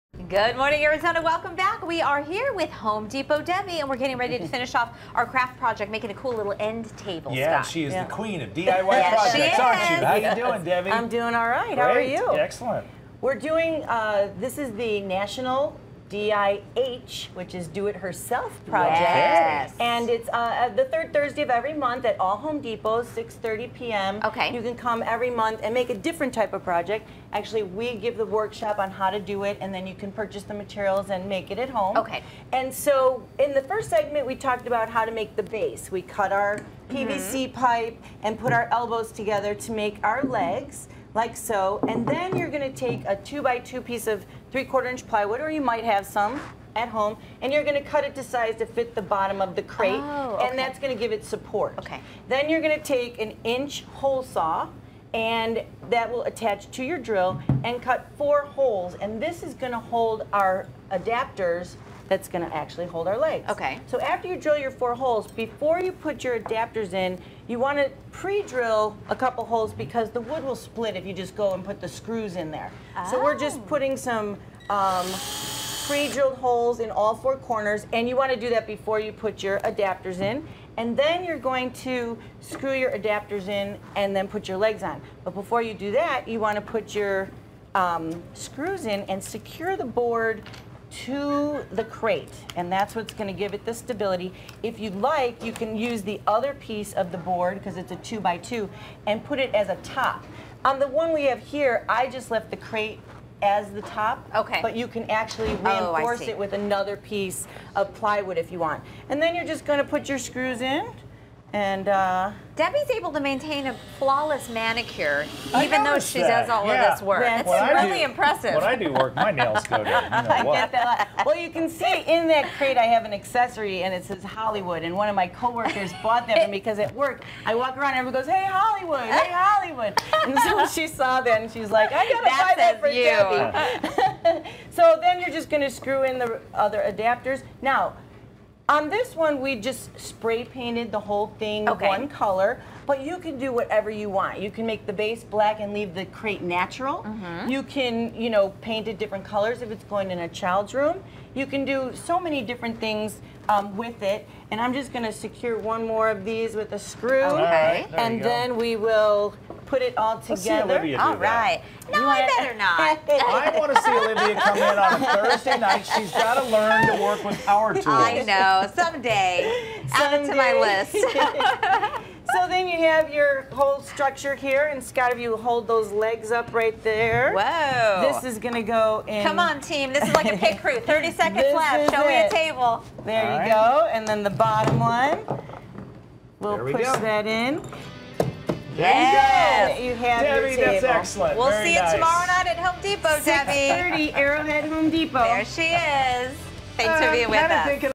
Do it Herself: Crate End Table
Debbie Hernandez from Home Depot shows us how to make an end table from an old crate. You can find out how by attending one of Home Depot's Do it Herself classes every third Thursday. for locations.